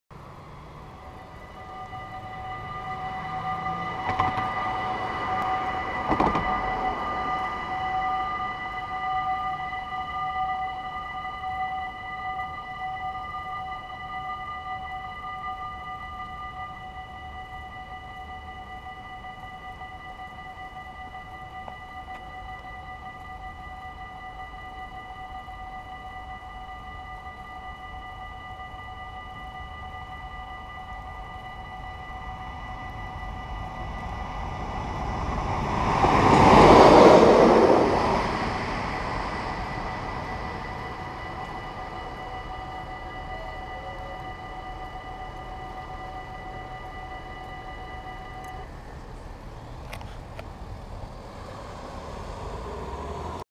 Spoorwegovergang Emmen // Ducth railroad crossing
Locatie: Nijbracht
Traject: Emmen-Zwolle
Soort: AHOB
Rode lichten: 12
Bellen: 7
Bomen: 6
Andreaskruisen: 5
Passeren:
- DB D-loc naar Emmen (vervolgens Emmtec)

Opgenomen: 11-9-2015